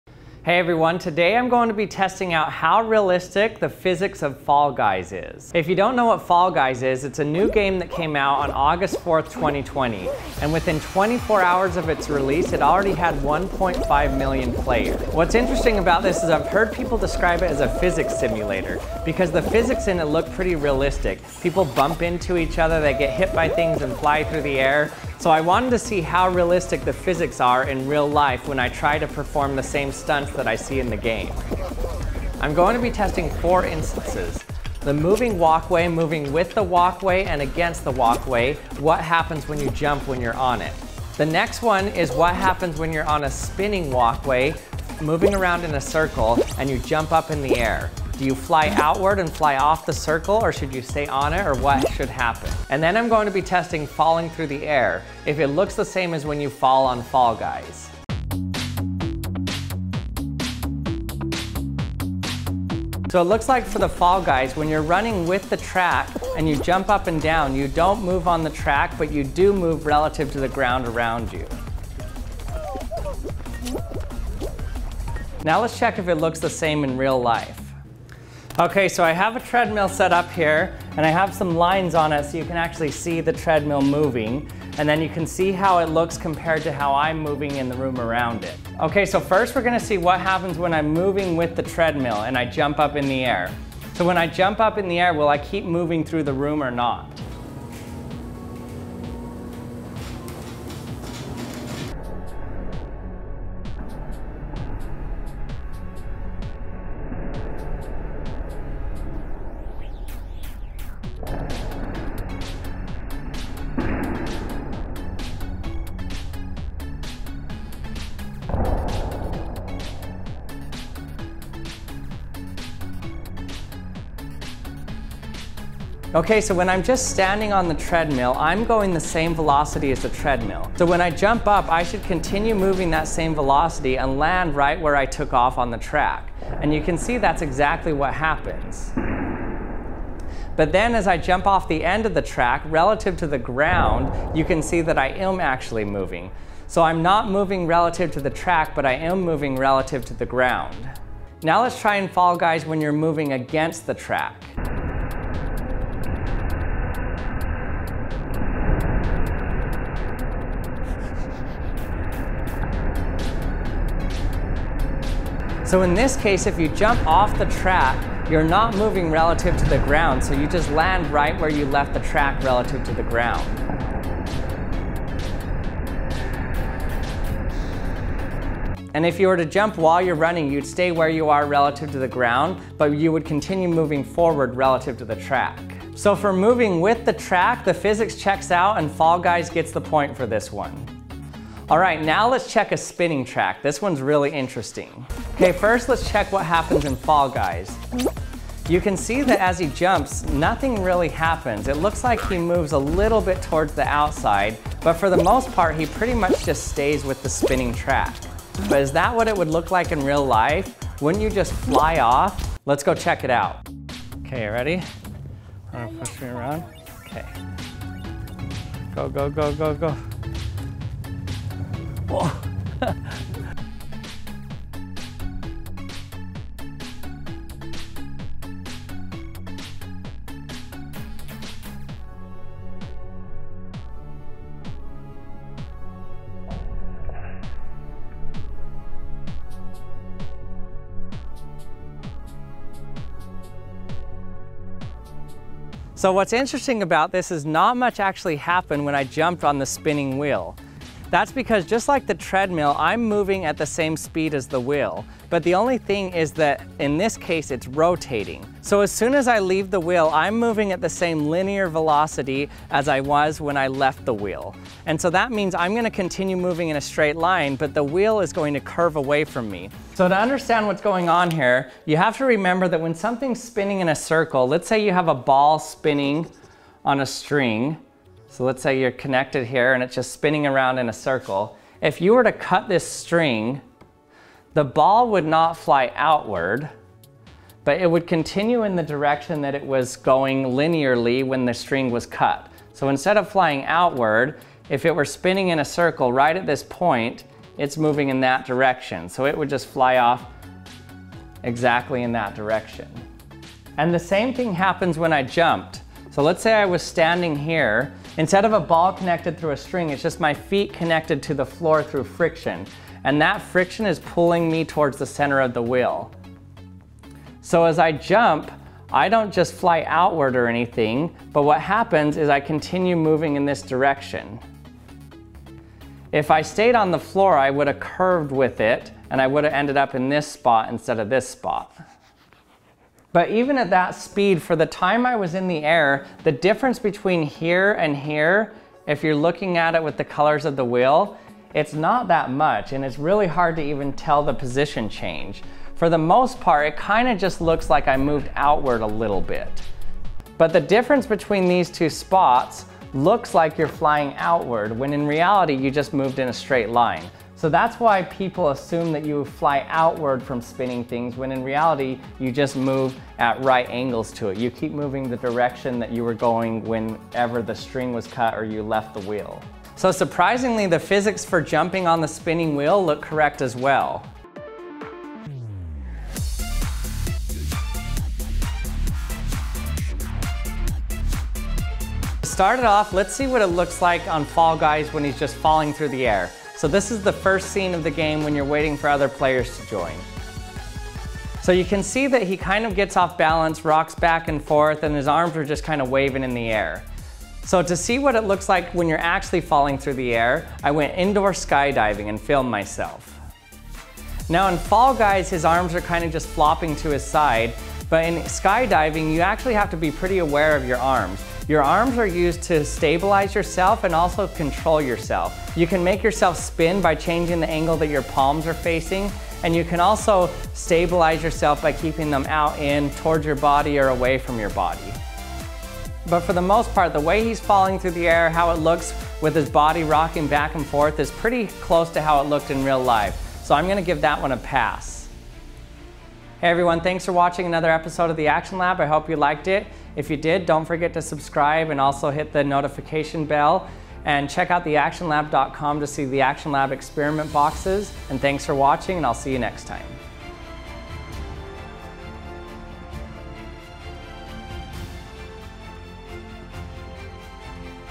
How Realistic Is The Physics In Fall Guys?
In this video I recreate the stunts in fall guys to see how realistic the physics are in the game. For example, what happens when you jump on a big spinning wheel? Do you fly off, or land at the same point the you jumped from?